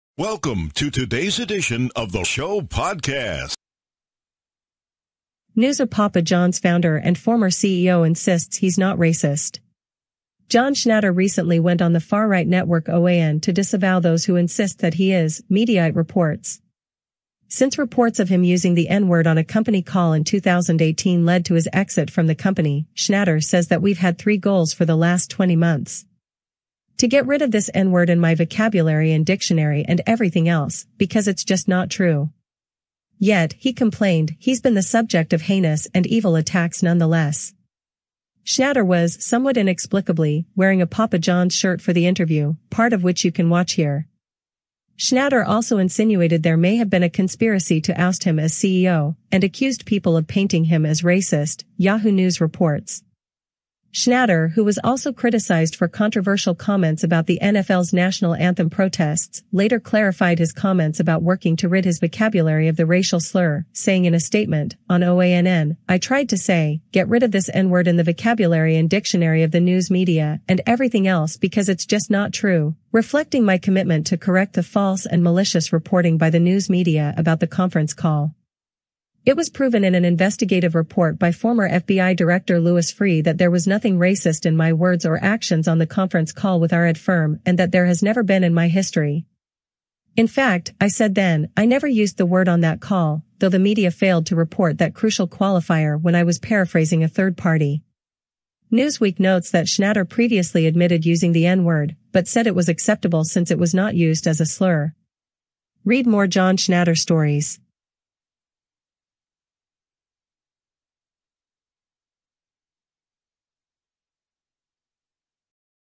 The Papa John's N-Word Saga Continues
(NEWSER)  Papa John's founder and former CEO insists he's not racist. John Schnatter recently went on the far-right network OAN to disavow those who insist that he is, Mediaite reports. Since reports of him using the n-word on a company call in 2018 led to his exit from the company, Schnatter says that "Weve had three goals for the last 20 months: To get rid of this n-word in my vocabulary and dictionary and everything else, because its just not true." Yet, he complained, he's been the subject of "heinous" and "evil" attacks nonetheless. Schnatter was, somewhat inexplicably, wearing a Papa John's shirt for the interview, part of which you can watch here. Schnatter also insinuated there may have been a "conspiracy" to oust him as CEO, and accused people of "painting" him as racist, Yahoo News reports.. . . . Schnatter, who was also criticized for controversial comments about the NFL's national anthem protests, later clarified his comments about working to rid his vocabulary of the racial slur, saying in a statement, "On OANN, I tried to say, Get rid of this n-word in (the) vocabulary and dictionary (of the news media), and everything else because its just not true,reflecting my commitment to correct the false and malicious reporting by the news media about the conference call. It was proven in an investigative report by former FBI Director Louis Freeh that there was nothing racist in my words or actions on the conference call with our ad firm and that there has never been in my history. In fact, I said then, I never used the word on that call, though the media failed to report that crucial qualifier when I was paraphrasing a third party." Newsweek notes that Schnatter previously admitted using the n-word, but said it was acceptable since it was not used as a slur. (Read more John Schnatter stories.)